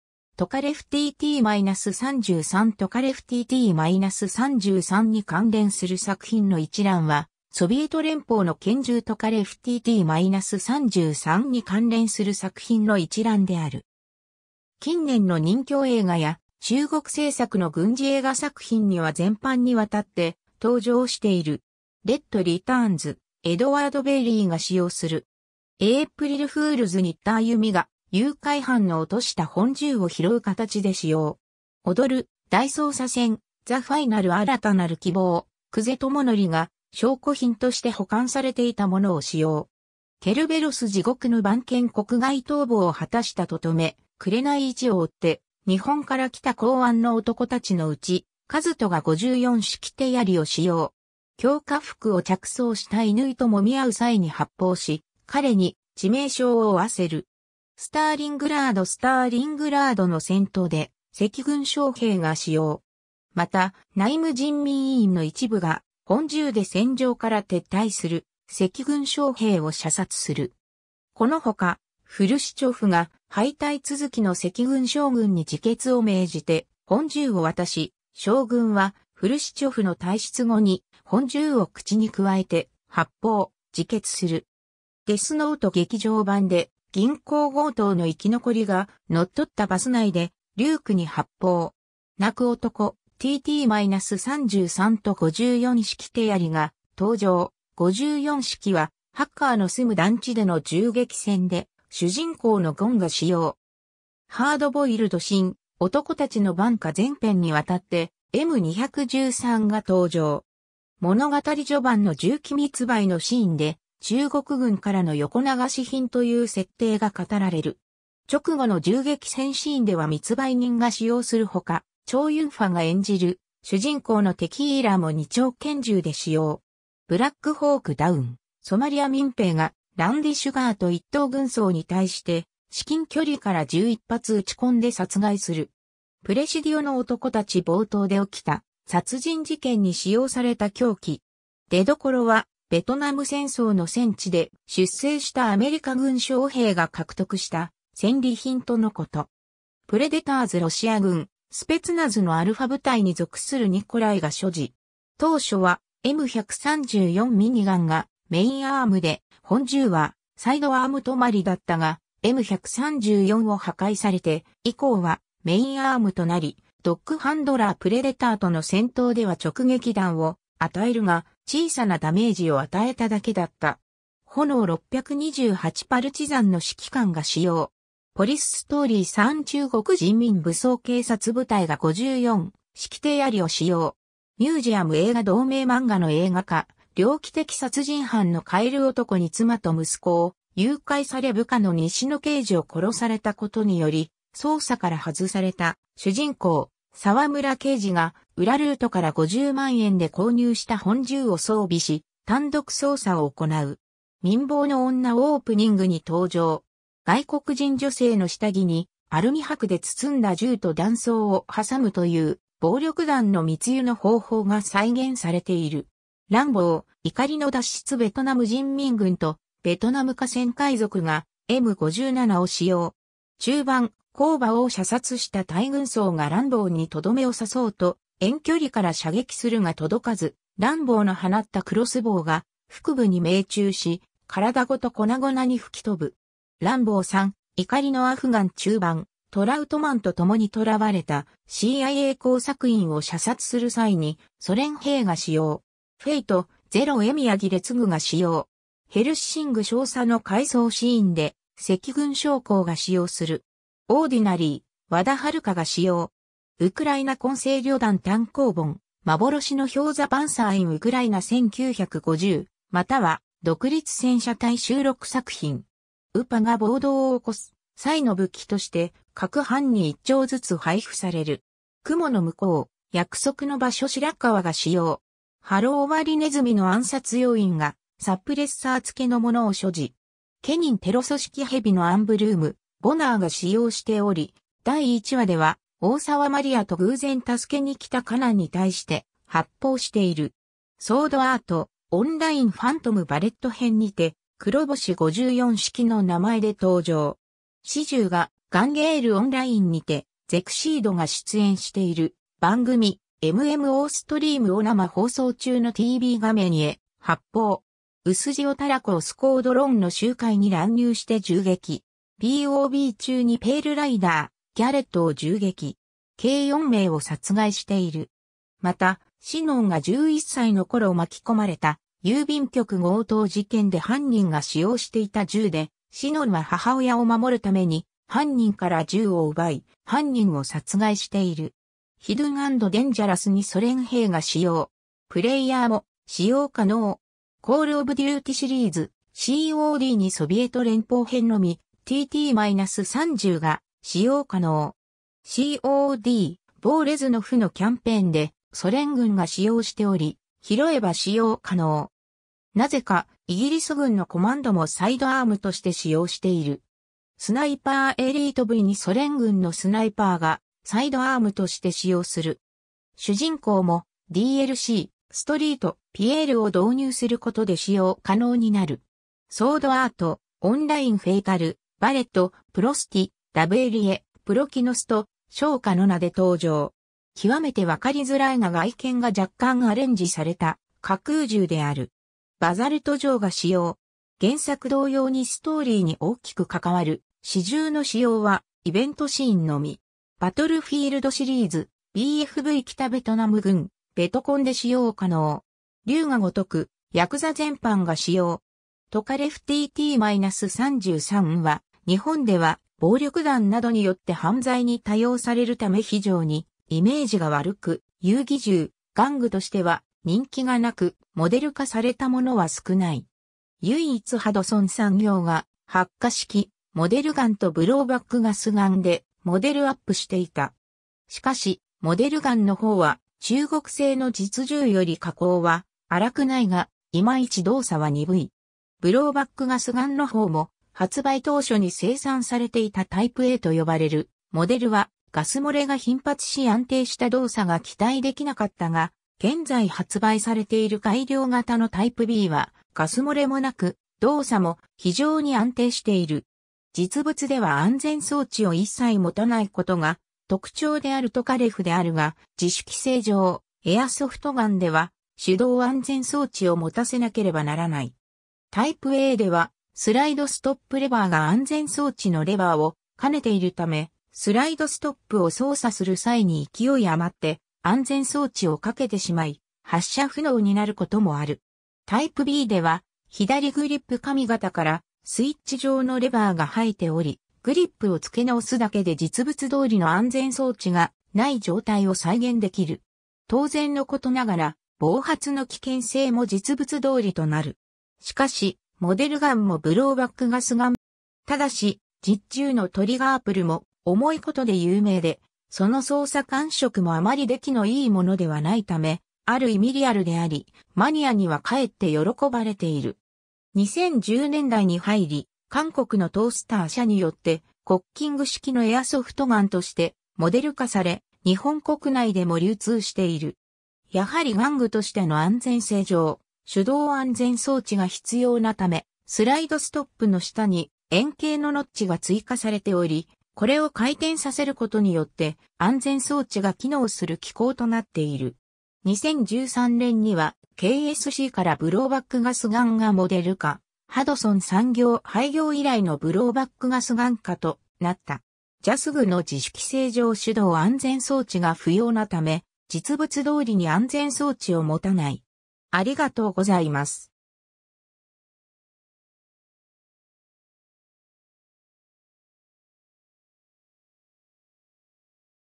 トカレフTT-33に関連する作品の一覧
トカレフTT-33（実物） トカレフTT-33に関連する作品の一覧（トカレフTT-33ににかんれんするさくひんのいちらん）は、ソビエト連邦の拳銃トカレフTT-33に関連する作品の一覧である。
 近年の任侠映画や、中国制作の軍事映画作品には全般に渡って登場している。
 『REDリターンズ』 エドワード・ベイリーが使用する。
 『エイプリルフールズ』 新田あゆみが、誘拐犯の落とした本銃を拾う形で使用。
 『踊る大捜査線 THE FINAL 新たなる希望』 久瀬智則が、証拠品として保管されていたものを使用。
 『ケルベロス-地獄の番犬』 国外逃亡を果たした都々目紅一を追って日本から来た公安の男たちのうち、一人が54式手槍を使用。
強化服を着装した乾ともみ合う際に発砲し、彼に致命傷を負わせる。
 『スターリングラード (2001年の映画)』 スターリングラードの戦闘で、赤軍将兵が使用。
また、内務人民委員の一部が本銃で戦場から撤退する赤軍将兵を射殺する。
この他、フルシチョフが敗退続きの赤軍将軍に自決を命じて本銃を渡し、将軍はフルシチョフの退室後に本銃を口に銜えて発砲、自決する。
 『デスノート』 劇場版で銀行強盗の生き残りが、乗っ取ったバス内でリュークに発砲。
 『泣く男』 TT-33と54式手槍が登場、54式はハッカーの住む団地での銃撃戦で主人公のゴンが使用。
 『ハード・ボイルド 新・男たちの挽歌』 全編に渡ってM213（民間市場向けの54式手槍）が登場。
物語序盤の銃器密売のシーンで中国軍からの横流し品という設定が語られる。
直後の銃撃戦シーンでは密売人が使用する他、チョウ・ユンファが演じる主人公のテキーラも二丁拳銃で使用。
 『ブラックホーク・ダウン』 ソマリア民兵がランディ・シュガート一等軍曹に対して至近距離から11発撃ちこんで殺害する。
 『プレシディオの男たち』 冒頭で起きた殺人事件に使用された凶器。
出所はベトナム戦争の戦地で、出征したアメリカ軍将兵が獲得した戦利品とのこと。
 『プレデターズ』 ロシア軍スペツナズのアルファ部隊に属するニコライが所持。
当初はM134 ミニガンがメインアームで、本銃はサイドアーム止まりだったが、M134を破壊されて以降はメインアームとなり、ドッグ・ハンドラー・プレデターとの戦闘では直撃弾を与えるが、小さなダメージを与えただけだった。
 『炎628』 パルチザンの指揮官が使用。
 『ポリス・ストーリー3』 中国人民武装警察部隊が54式手槍を使用。
 『ミュージアム 映画』 同名漫画の映画化、猟奇的殺人犯のカエル男(妻夫木聡)に妻と息子を誘拐され部下の西野刑事(野村周平)を殺された事により捜査から外された主人公沢村刑事(小栗旬)が裏ルートから50万円で購入した本銃を装備し単独捜査を行う。
 『ミンボーの女』 オープニングに登場。
外国人女性の下着にアルミ箔で包んだ銃と弾倉を挟むという、暴力団の密輸の方法が再現されている。
 『ランボー/怒りの脱出』 ベトナム人民軍とベトナム河川海賊がM57を使用。
中盤、コー・バオを射殺したタイ軍曹がランボーにとどめを刺そうと遠距離から射撃するが届かず、ランボーの放ったクロスボウが腹部に命中し、体ごと粉々に吹き飛ぶ。
 『ランボー3/怒りのアフガン』 中盤、トラウトマンと共に囚われたCIA工作員を射殺する際にソ連兵が使用。
 『Fate/Zero』 衛宮切嗣が使用。
 『HELLSING』 少佐の回想シーンで、赤軍将校が使用する。
 『Ordinary±』 和田晴夏が使用。
 『ウクライナ混成旅団』 単行本「幻の豹 The Panther in Ukraina 1950」または「独立戦車隊」収録作品。
UPAが暴動を起こす際の武器として、各班に1丁ずつ配布される。
 『雲のむこう、約束の場所』 白川が使用。
 『ハロー張りネズミ』 КГБの暗殺要員が、サプレッサー付の物を所持（発砲はしない）。
 　『CANAAN』 テロ組織蛇のアンブルーム、ボナーが使用しており第一話では大沢マリアと偶然助けに来たカナンにたいして発砲している。
 　　『ソードアート·オンライン』 ファントム·バレット編にて「黒星54式」の名前で登場。
 死銃(デス·ガン)がガンゲイル·オンライン(ＧＧＯ)にてゼクシード(XeXeeD)が出演している番組「ＭＭＯストリーム」を生放送中のTV画面ヘ発砲。
薄塩たらこをスコードロンの集会に乱入して銃撃。
BoB(バレット·オブ·バレッ...